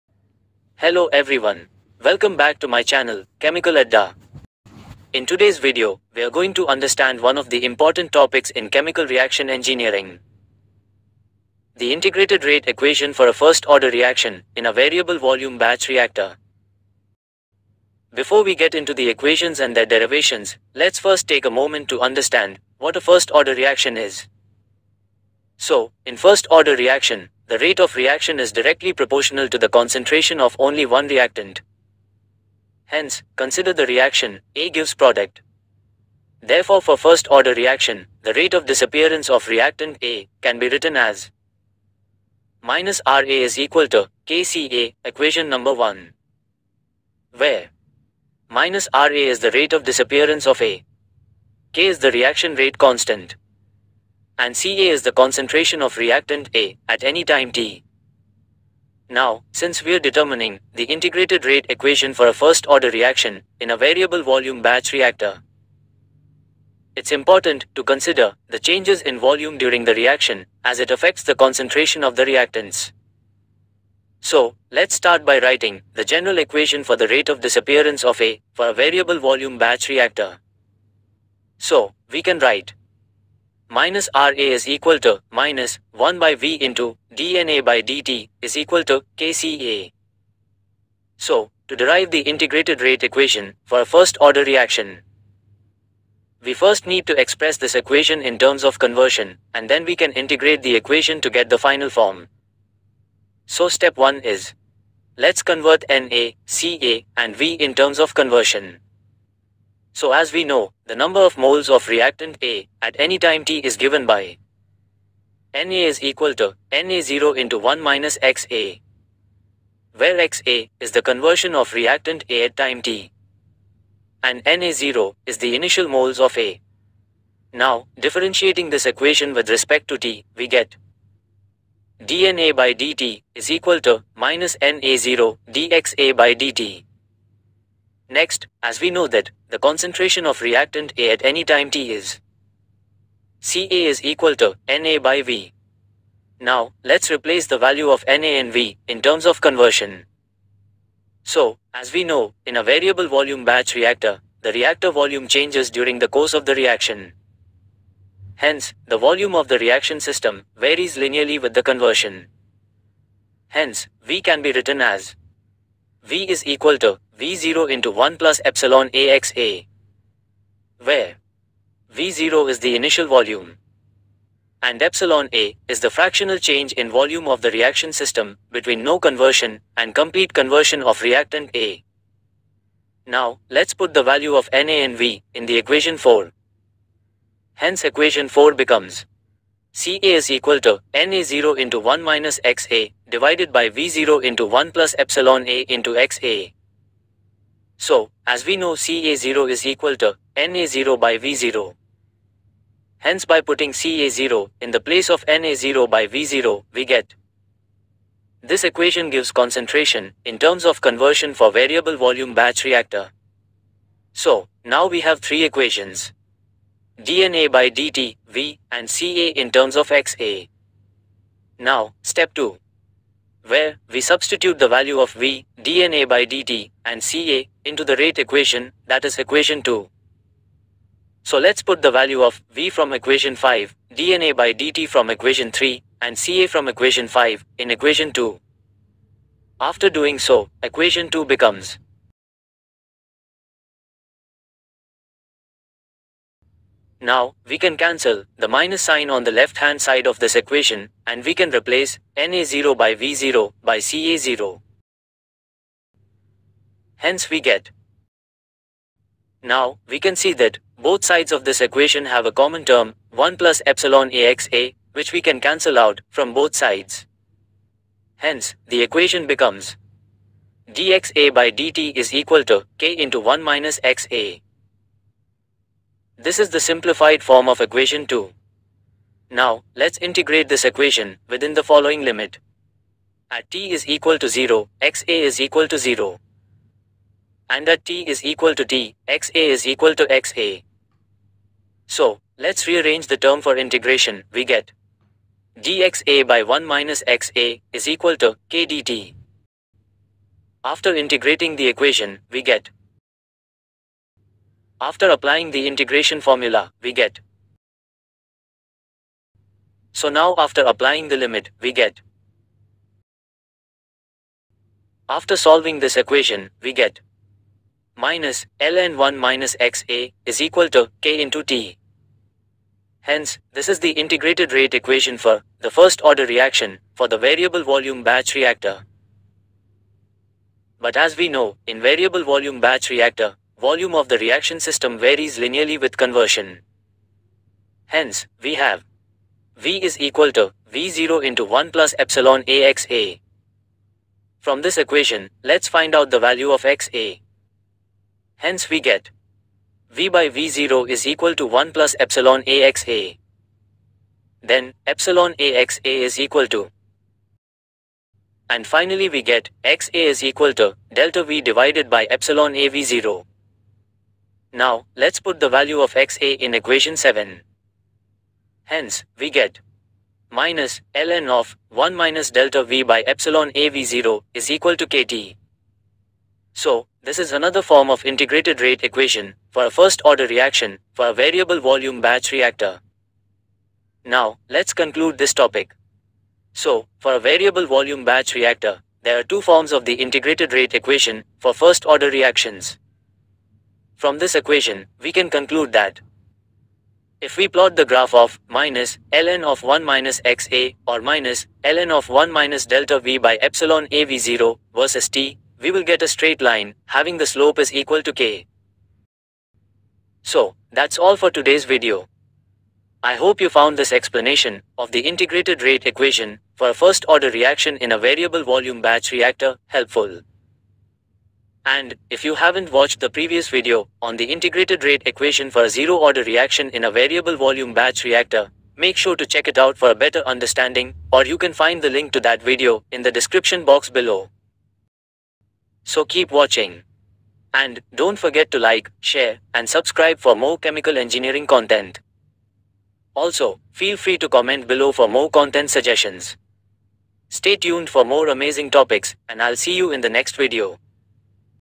Variable Volume Batch Reactor | Integrated Rate Equation for First Order Reaction
Hello friends in this video we will discuss
What is the Integration Rate Equation for First Order Reaction For Variable Volume Batch Reactor
Variable volume batch reactor
@Chemical adda

Link of video Variable volume batch reactor